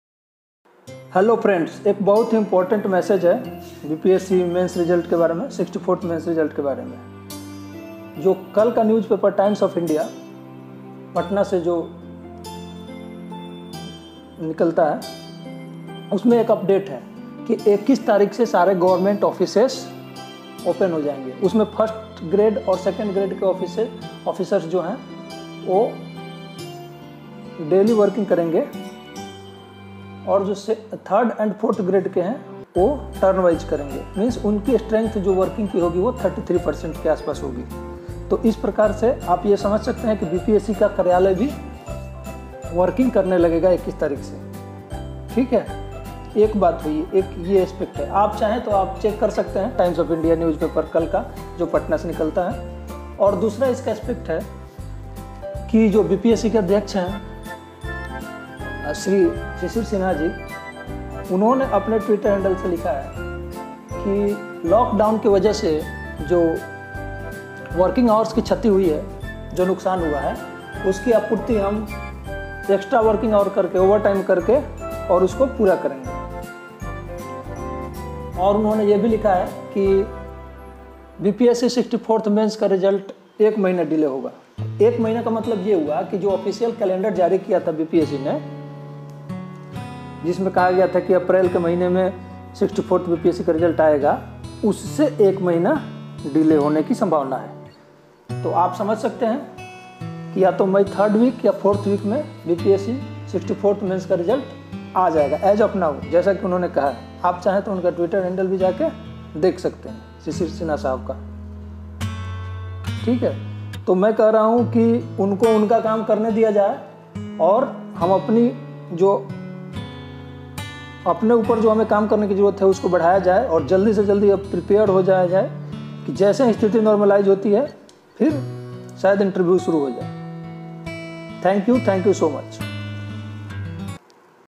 64TH BPSC MAINS RESULT DATE UPDATE
THIS VIDEO IS ABOUT INFORMATION ON 64TH BPSC MAINS RESULT EXPECTED DATE.
इसमें ६४ वी BPSC MAINS RESULT  के बारे में जानकारी दी गई है।